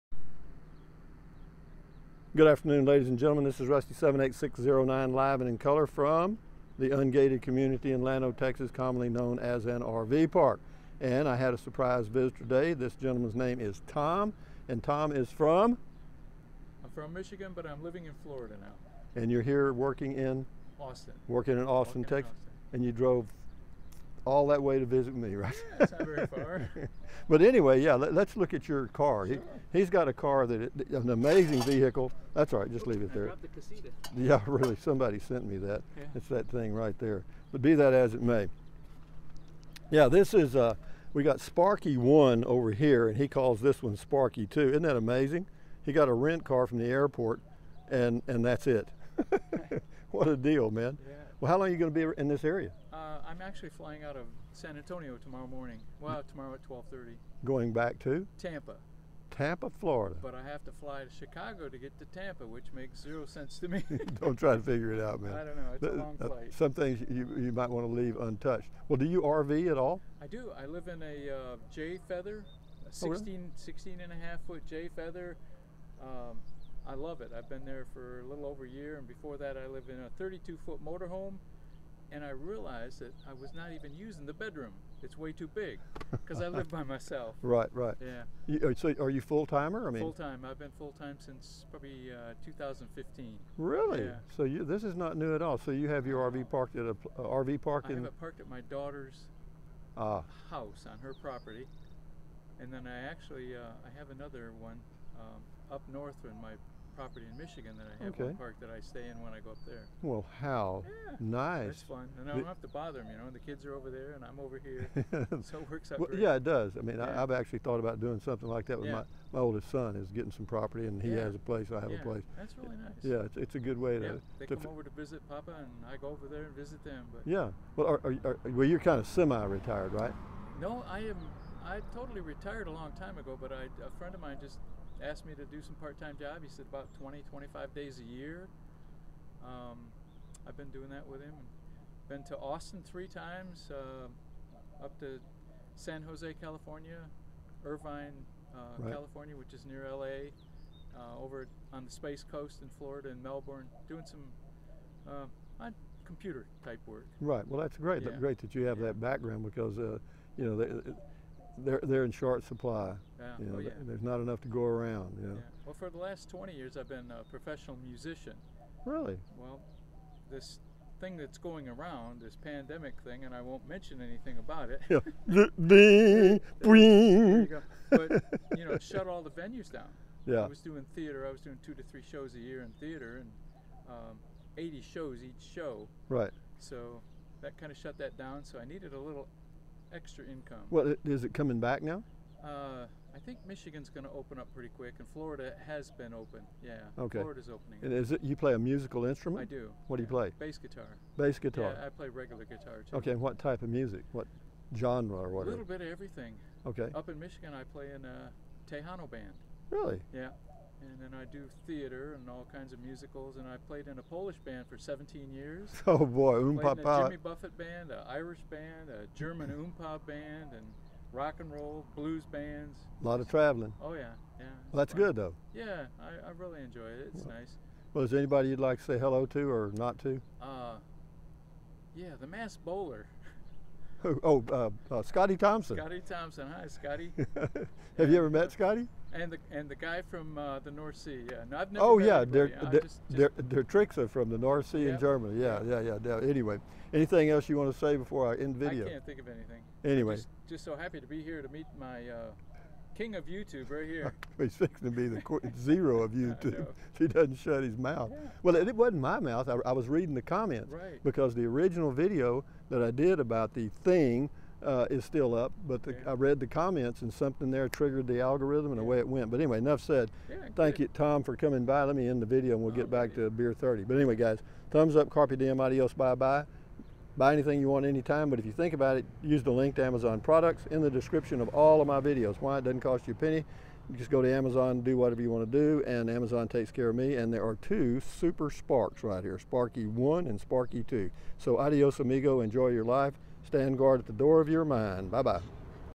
MEET TOM...FULL TIME RV'er...and Sparky II
Tom came by for a visit at BEER THIRTY. He's from Tampa Fla...doing some work in the Austin Tx area. thanks!!!